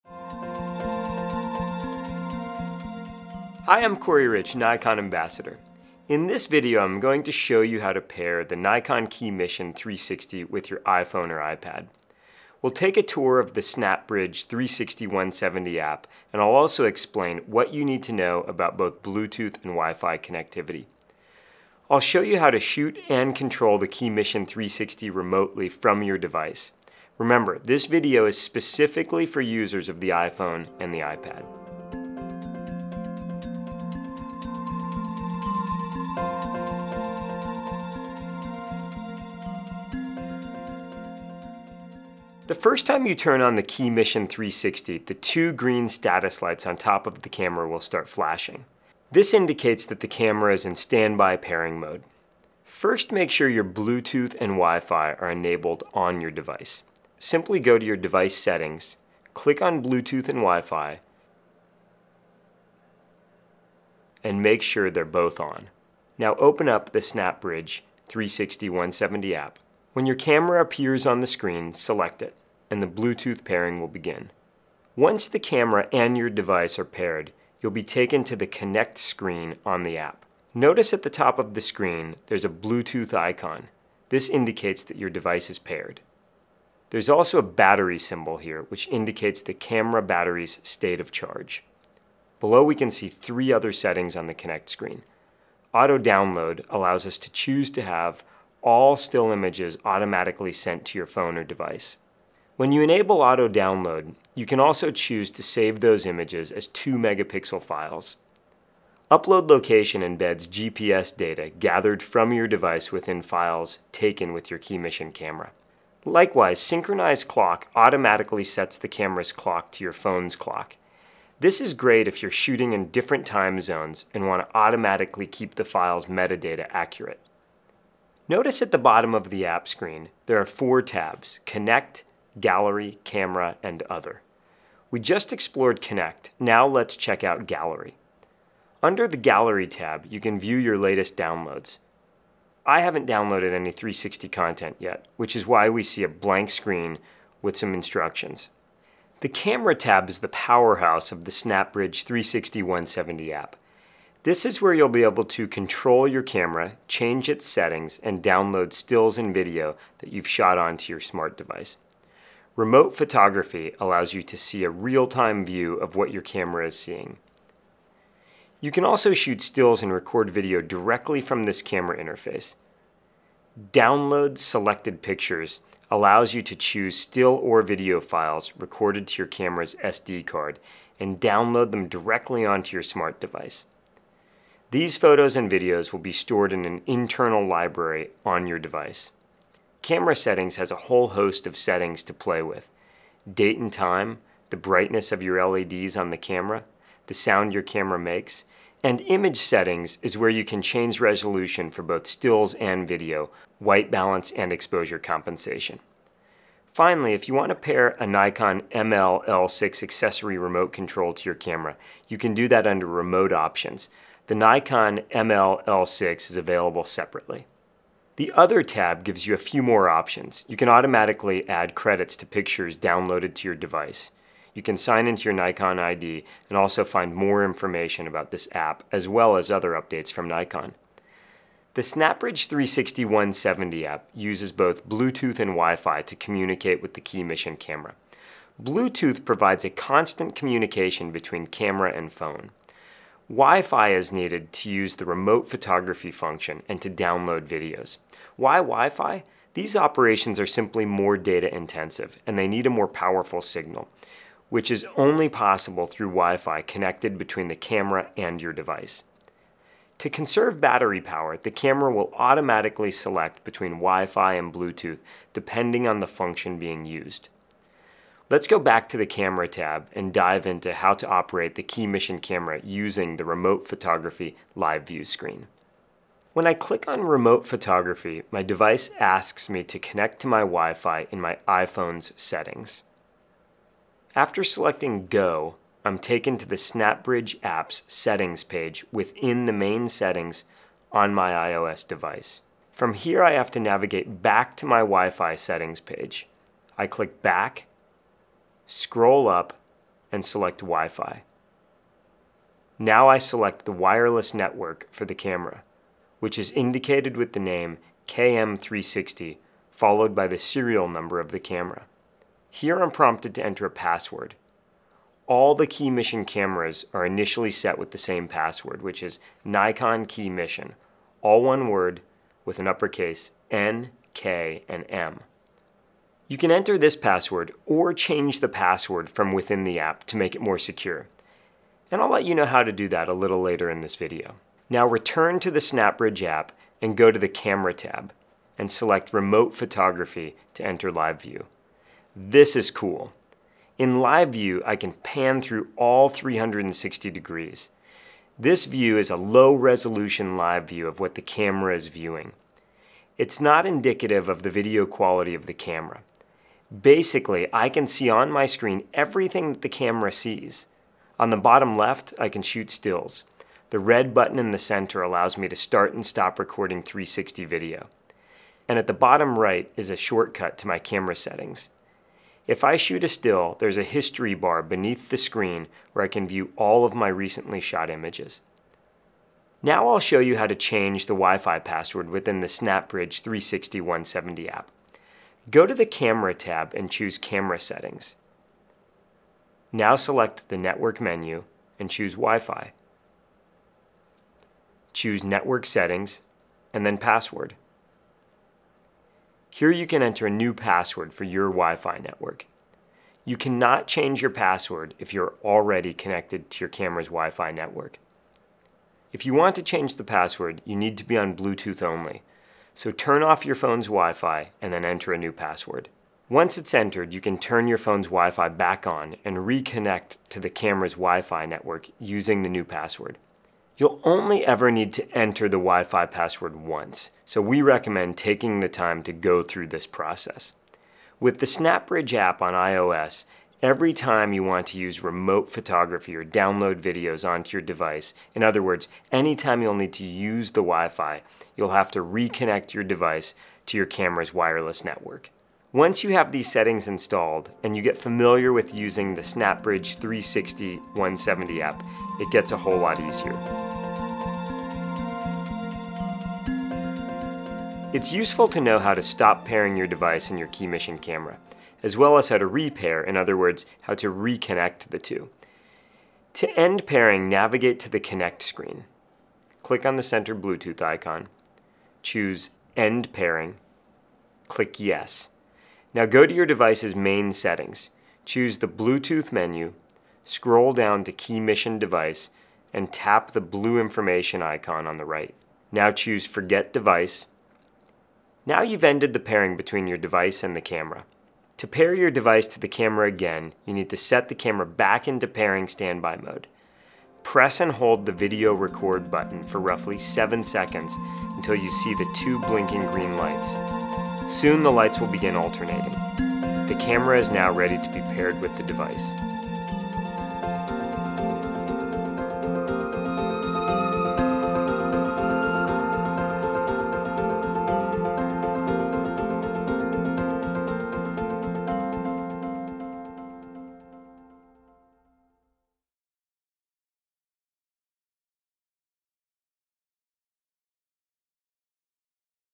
Pairing KeyMission 360 with iPhone or iPad
In this video, you’ll learn how to pair your KeyMission 360 with an iPhone or iPad, take a tour of the SnapBridge 360/170 application, and see how to operate the camera remotely from your device. Read more about the KeyMission 360 on our